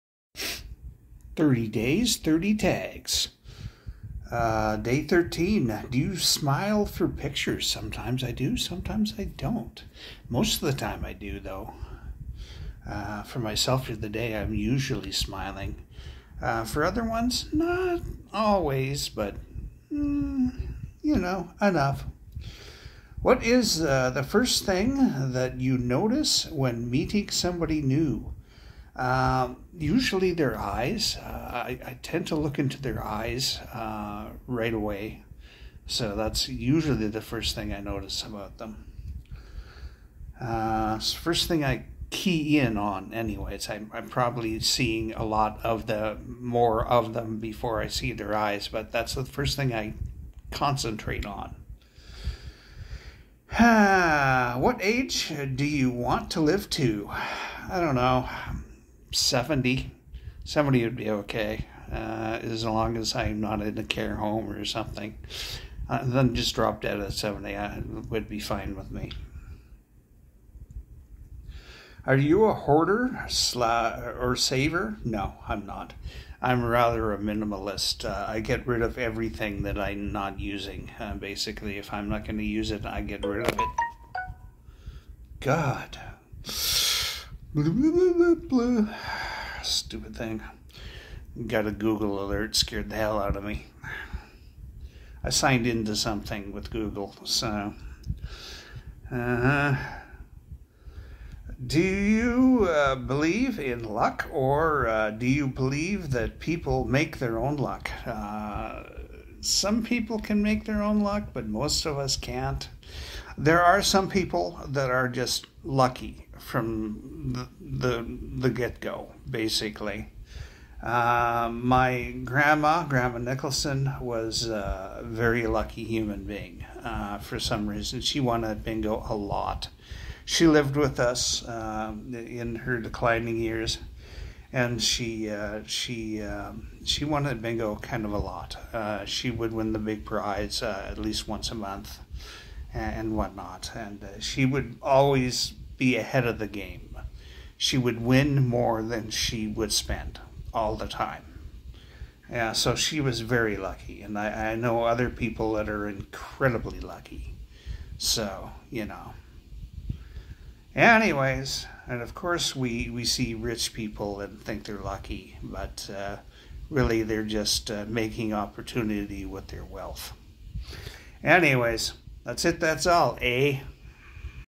Day 13
1. Do you smile for pictures?
2. What is the first thing that you notice when meeting someone new?
3. What age do you want to live to?
4. Are you a hoarder or saver?
5. Do you believe in luck, or do you believe that people make their own luck?